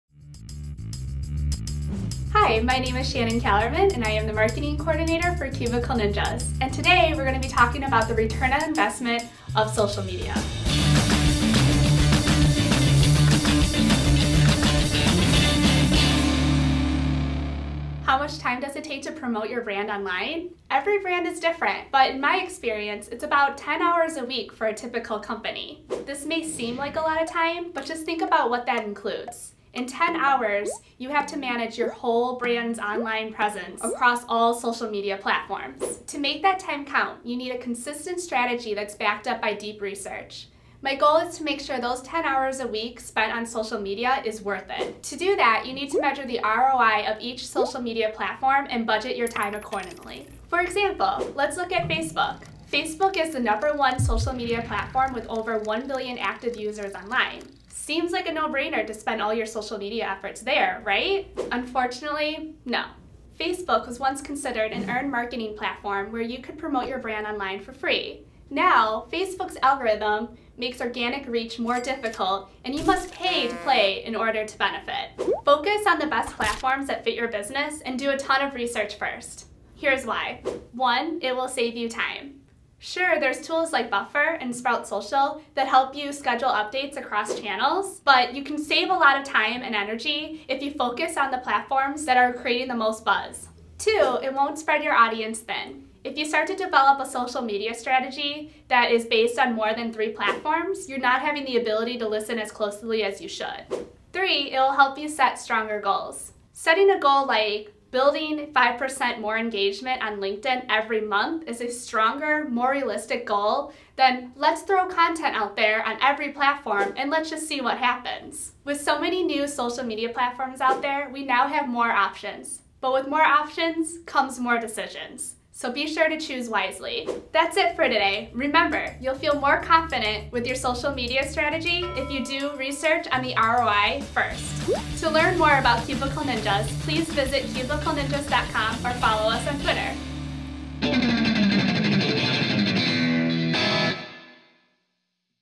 Ideas From the Dojo - Episode 3 - Measuring the ROI of Social Media
Let's face it, measuring the ROI of social media is tough! But developing a social media strategy the right way will help you measure how well you're doing on social media.

Here's the overview!

DO: Narrow your strategy down and focus on platforms that best fit your business.

Why?

1. It Will Save You Time
2. It Won't Spread Your Audience Thin
3. It Will Help You Set Stronger Goals